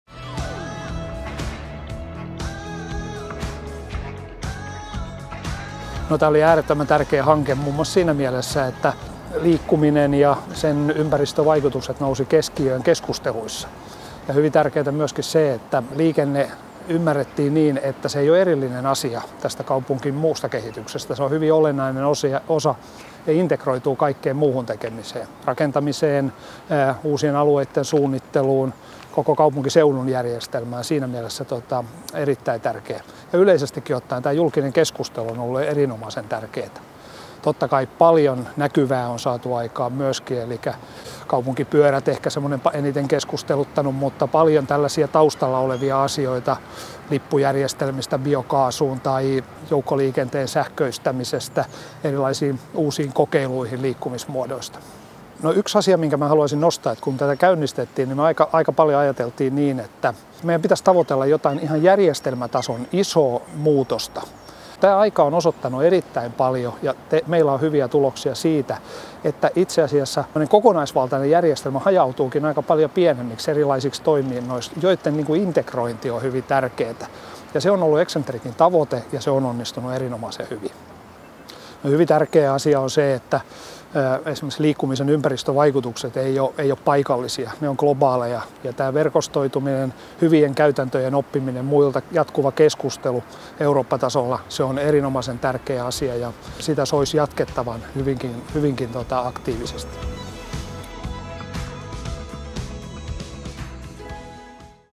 CIVITAS ECCENTRIC in Turku
The CIty of Turku tested a variety of innovative sustainable mobility solutions during the four years of the CIVITAS ECCENTRIC project (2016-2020).